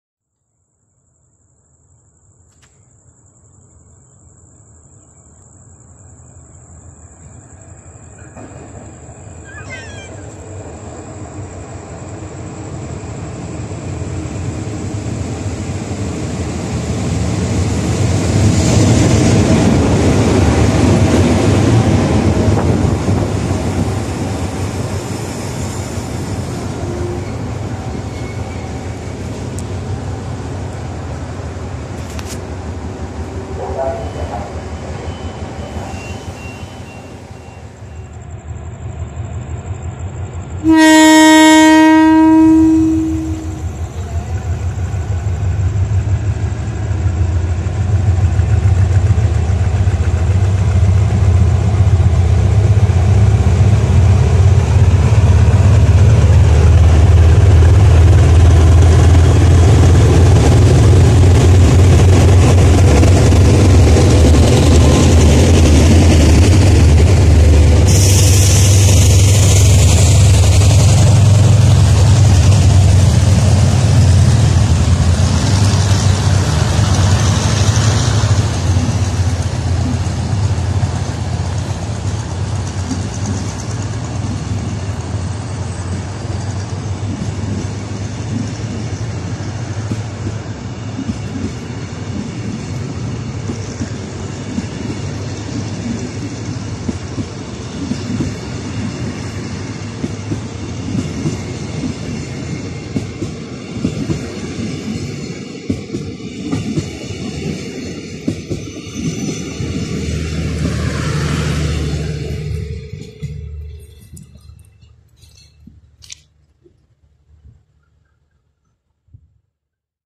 อีสานวัตนา 24 - Hitachi 4521 - สถานีศรีสะเกษ - 22 มิ.ย. 61 [Full HD]
ขบวนรถด่วนพิเศษอีสานวัตนาที่ 24 อุบลราชธานี-กรุงเทพ
รถจักรทำขบวน Hitachi 4521
ทำขบวนหยุดรับ-ส่งผู้โดยสาร สถานีศรีสะเกษ เวลา 20:00-20:03 น.